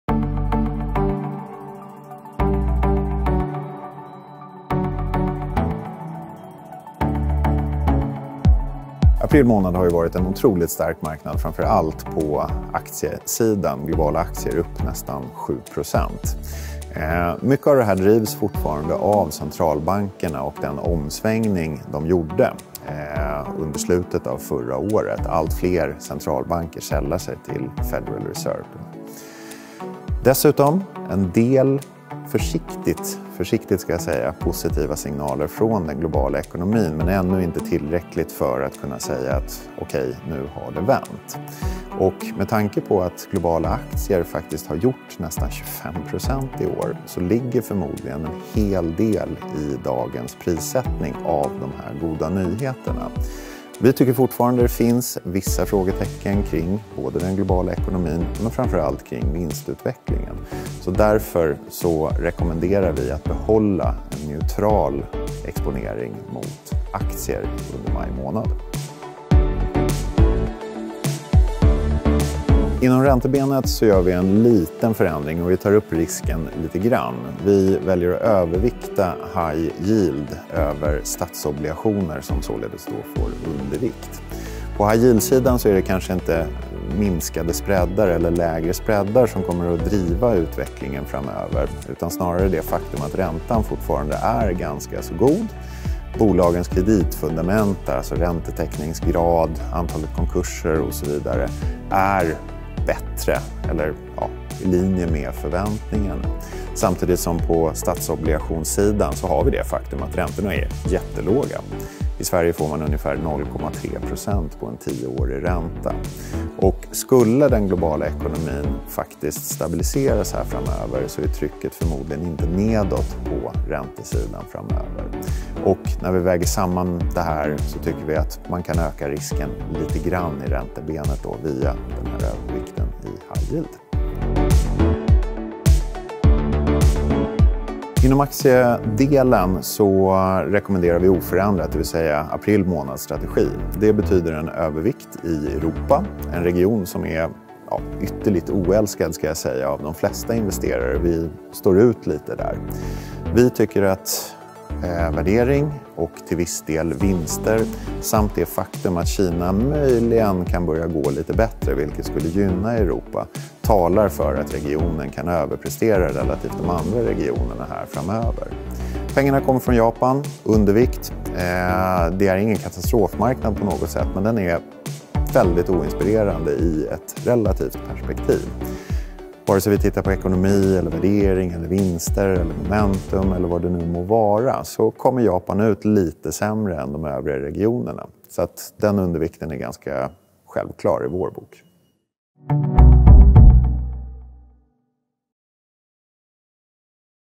Marknadssyn Maj 2019 | Nordea Sverige
Vår chefsstrateg Michael Livijn om strategin för allokeringen av tillgångar under maj 2019.